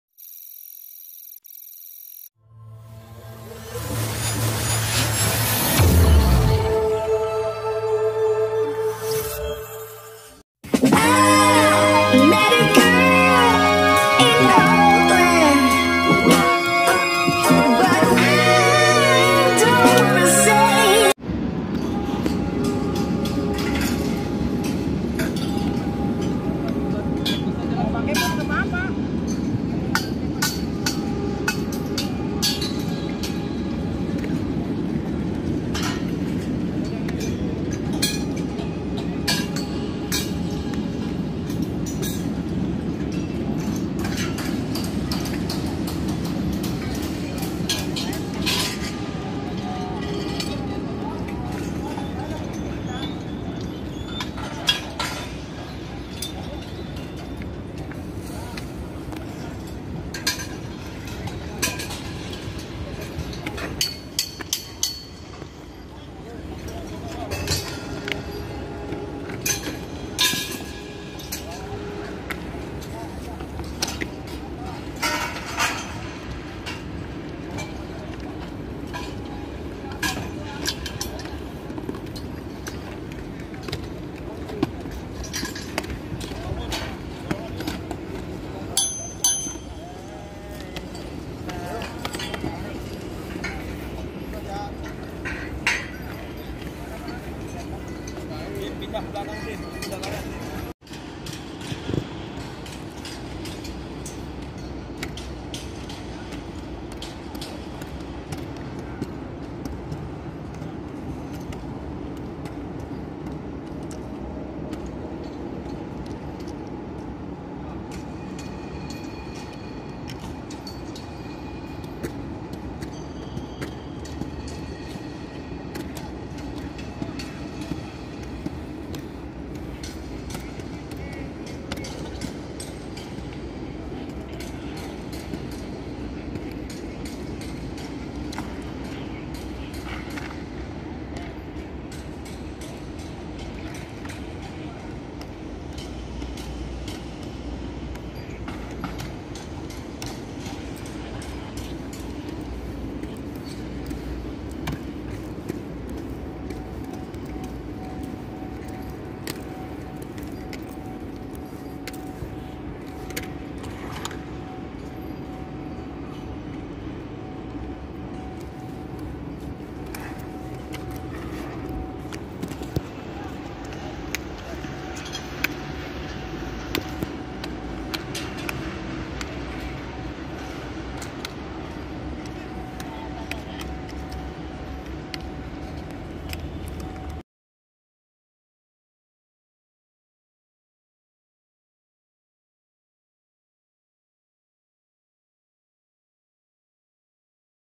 Rumput Hybrid JIS Ditutup Armordeck
Konser musik akan digelar di Jakarta Internasional Stadium. pemasangan Amordeck pun dilakukan yang akan membuat rumput menguning bahkan bisa jg mati.